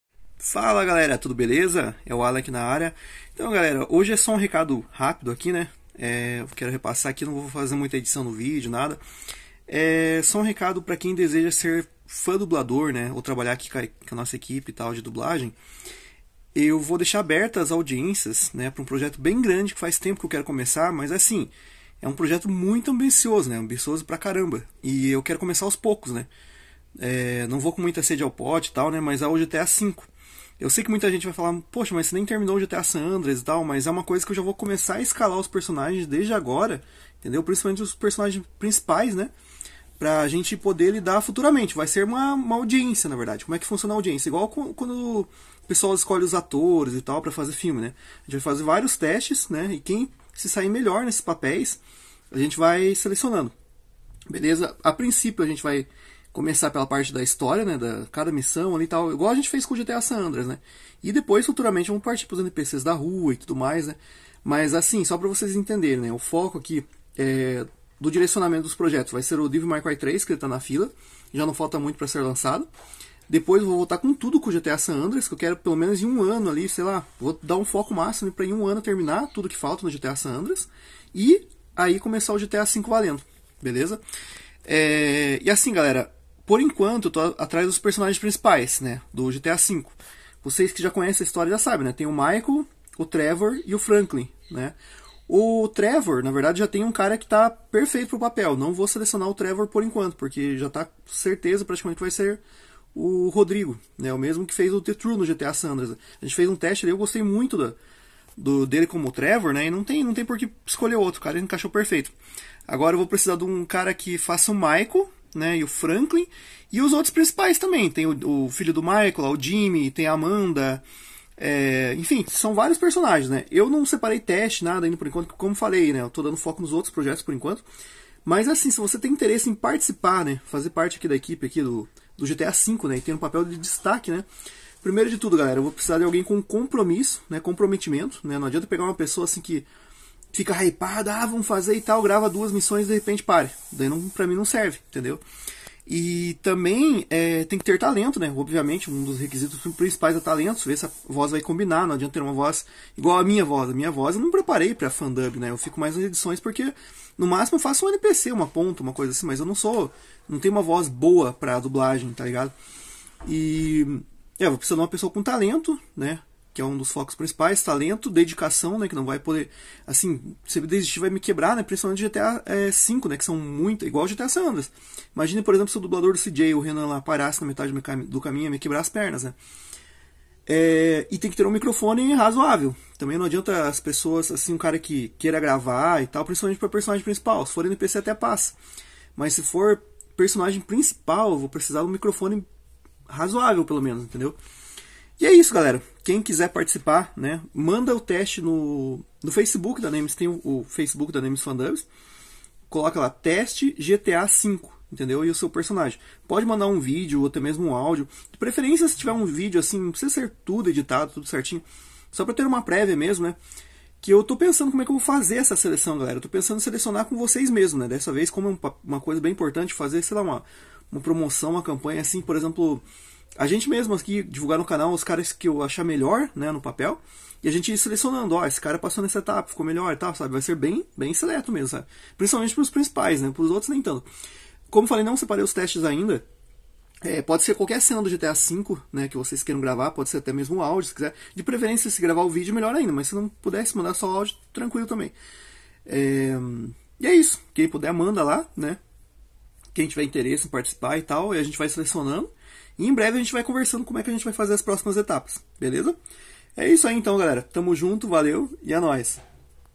RECADO IMPORTANTE AUDIÊNCIAS ABERTAS PARA GTA 5 DUBLADO! (VAMOS DUBLAR GTA 5 SÓ DEPOIS DO SA)
Quer participar do grande projeto do GTA 5 Dublado?
Envia seu teste no Facebook da Nemesis Fandubs
Que vamos conversando valeu galera TMJ